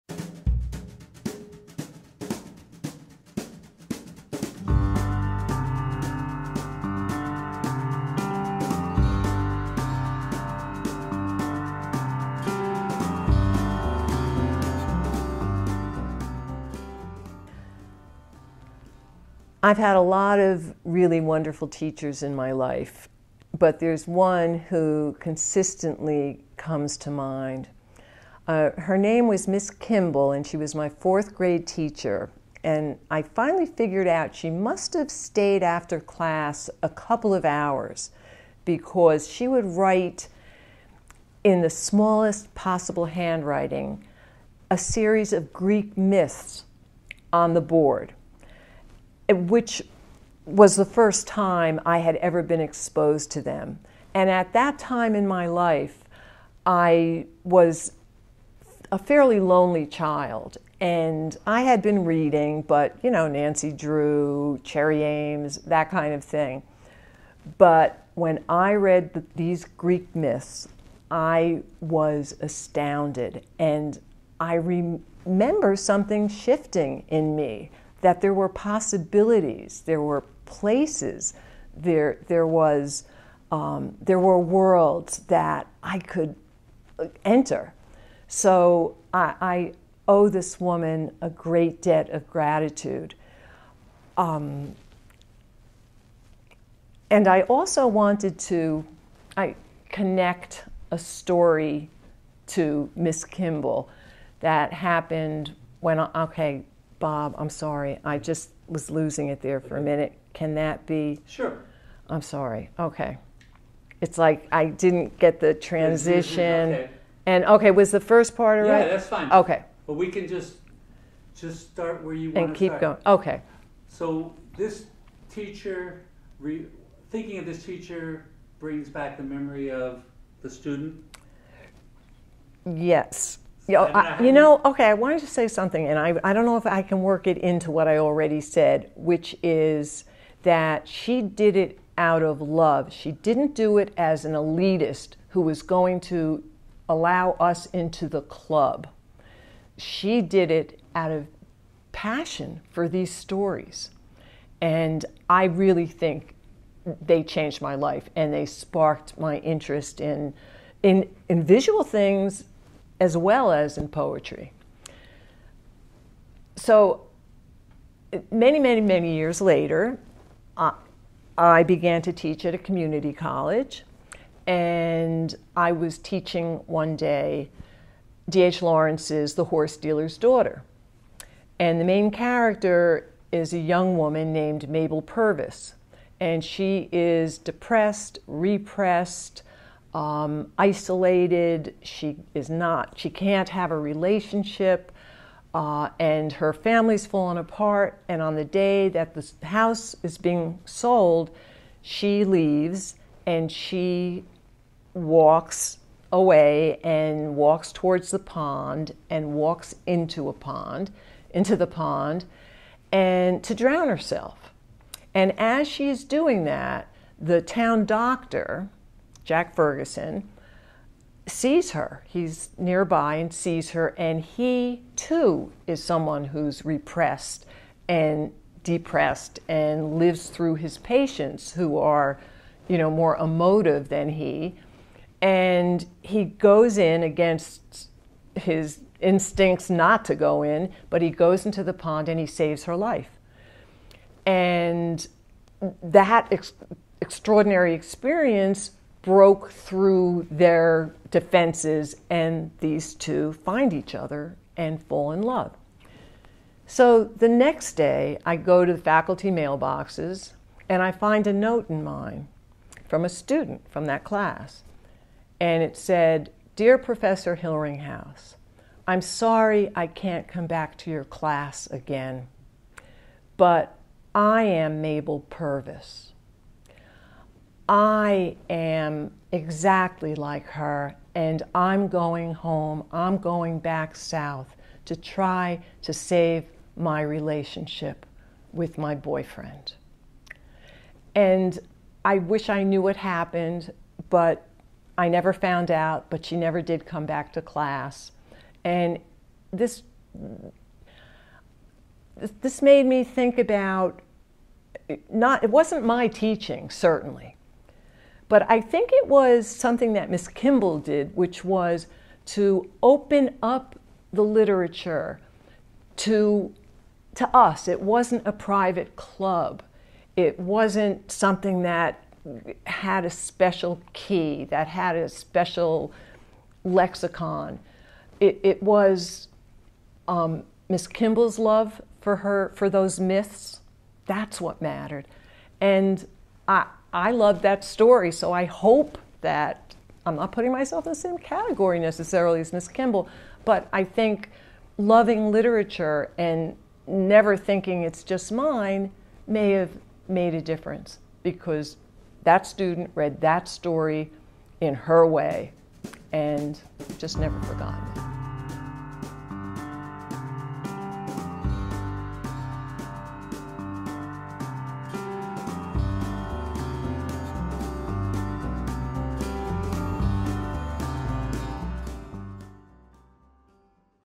Linda Hillringhouse - Teachers Make a Difference - Miss Kimble
Linda Hillringhouse is a self-taught painter who has shown her work at many local museums and galleries throughout the Tri-State area including the Newark and Paterson Museums, the Ben Shahn Center for Visual Arts at William Paterson University and the Weinstein Gallery at Fairleigh Dickinson University. She was selected for inclusion in the 20th century Self-Taught Artists Archive Collection at the Museum of American Folk Art, New Haven, CT. Hillringhouse holds an MFA from Columbia University in Poetry. She was a first-place winner of the Allen Ginsberg Poetry Award (2014) and second-place winner in Nimrod’s Pablo Neruda Prize for Poetry (2012). She has received fellowships from the Macdowell Colony, Yaddo, and the Virginia Center for the Creative Arts. Her work has appeared in Lips; New Ohio Review; Nimrod International Journal of Prose and Poetry; Oberon, Prairie Schooner; The Paterson Literary Review, and elsewhere. She has a book of poetry forthcoming from the New York Quarterly Press in 2020.